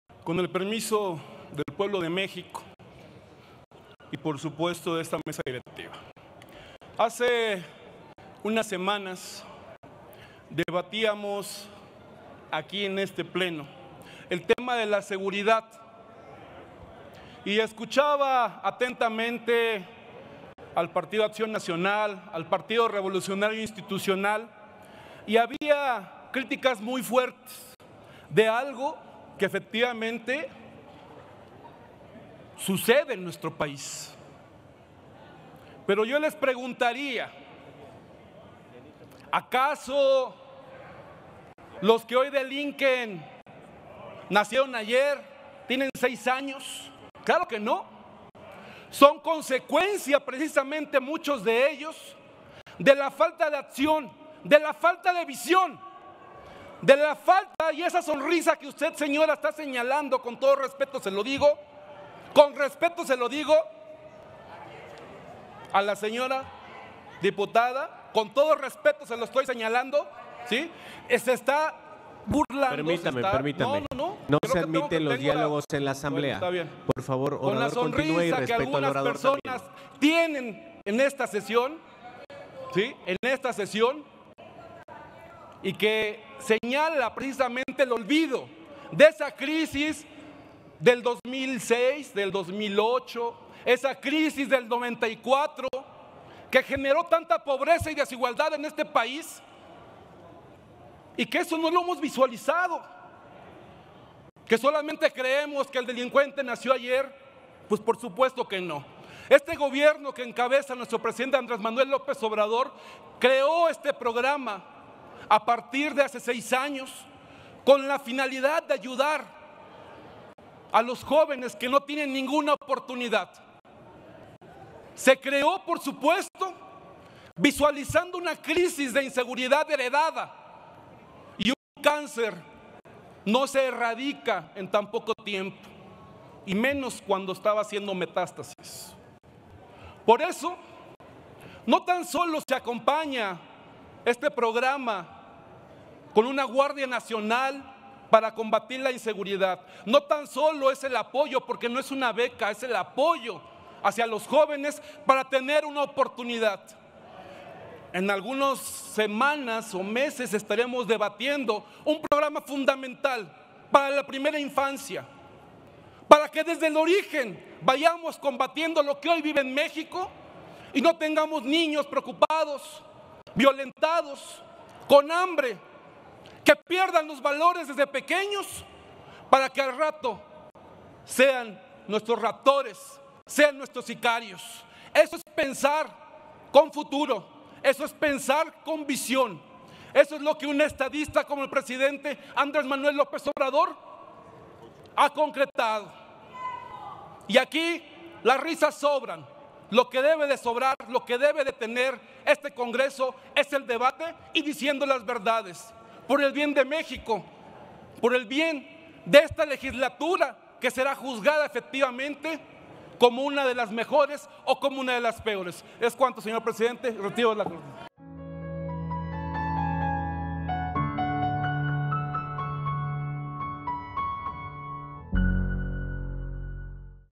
Dip. Juan Ángel Flores Bustamante (MORENA) / Presentación de reservas
El diputado Juan Ángel Flores Bustamante (MORENA) presenta reservas con el objetivo de modificar el dictamen que adiciona un segundo párrafo al artículo 123 de la Constitución Política, en materia de apoyo a jóvenes.